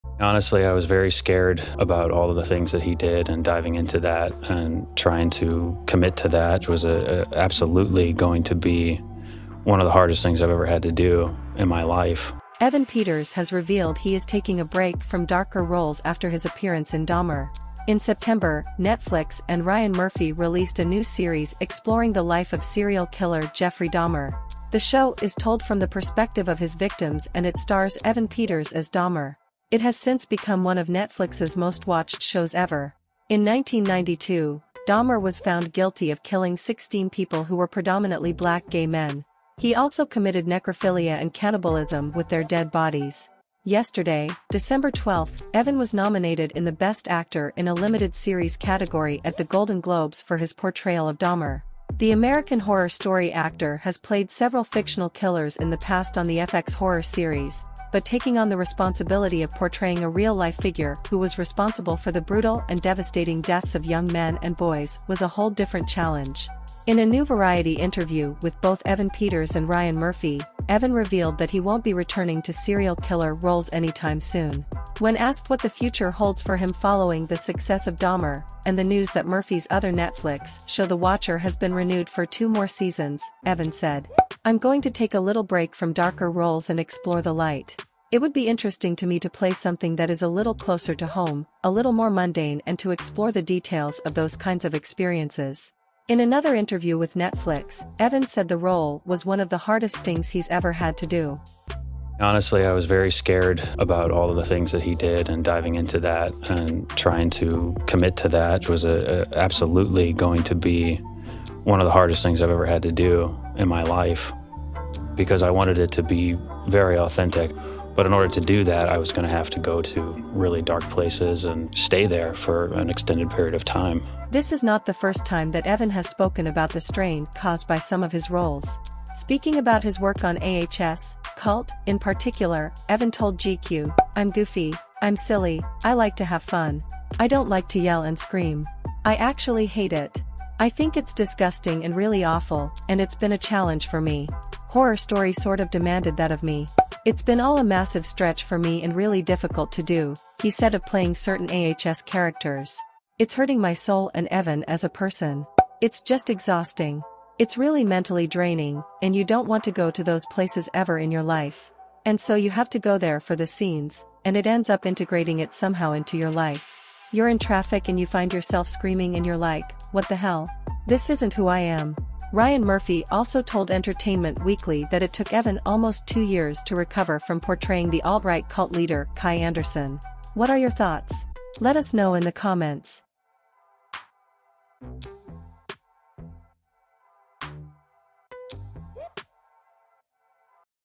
Evan Peters' Future in 'The Jeffrey Dahmer Story': Will He Return for Season 2?
Today We Have Evan Peters Who Has Shared That He Will Be Taking A Break From Darker Roles

👍Drop a LIKE for MORE Trending Topics